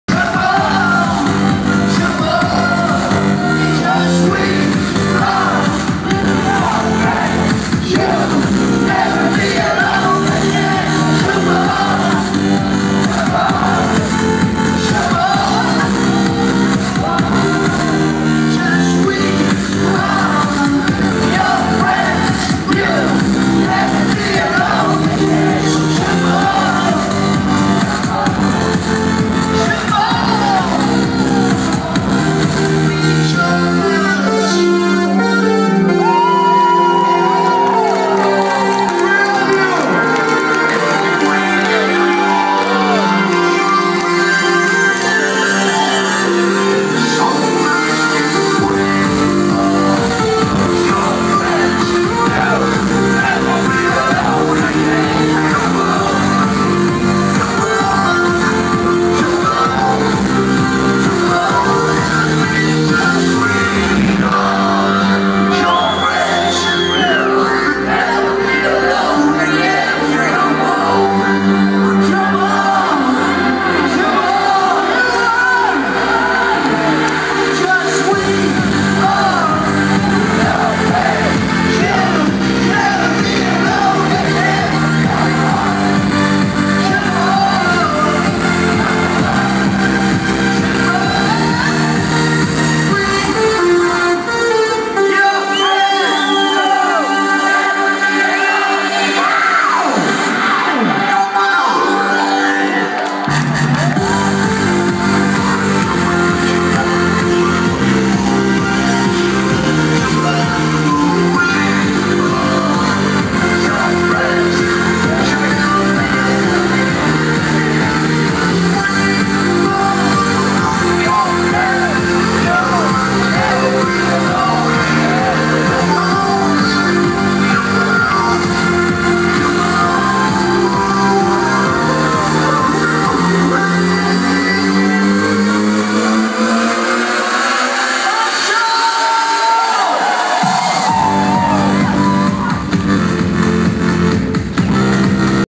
@A.G. Trio @ We Are Your Friends - FreeForm Festival 2012
Austriacy przedstawiciele electro-house na FFF 2012